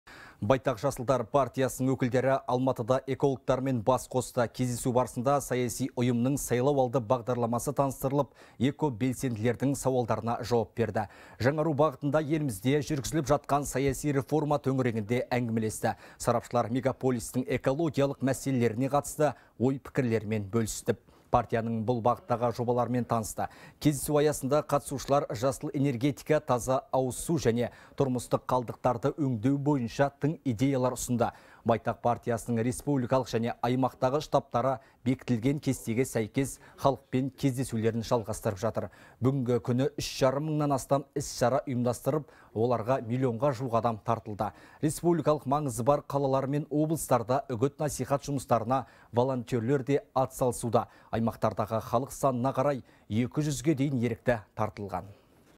«Байтақ» жасылдар партиясы Алматыда экологтармен бас қосты
Кездесу барысында саяси ұйымның сайлауалды бағдарламасы таныстырылып, экобелсенділердің сауалдарына жауап берді.